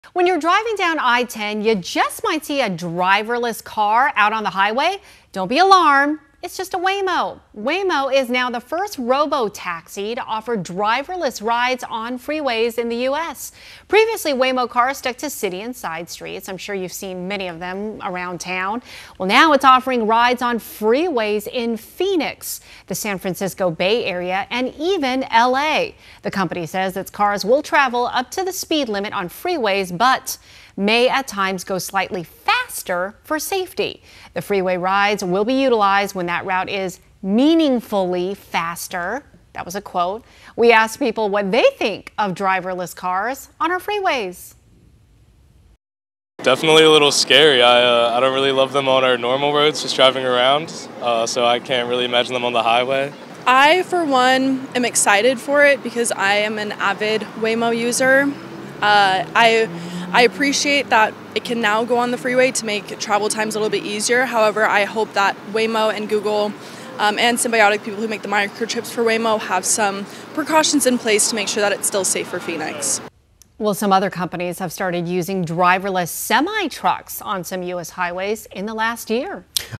Waymo cars will now be driving on Phoenix freeways
Waymo's autonomous cars are expected to start driving on freeways in Arizona and California.